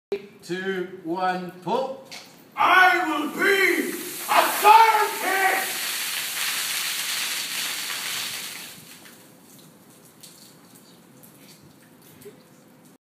Centrepoint tv ad Behind the Scenes 'The Experiment'
HERO created and produced a tv ad for Centrepoint the charity that supports homeless young people, we filmed in Camberwell with a group of young actors and a professional crew that all gave their time for nothing.
This scene shows the moment when we let off the fizzy drink bottles filled with mints and they exploded in synch with each other.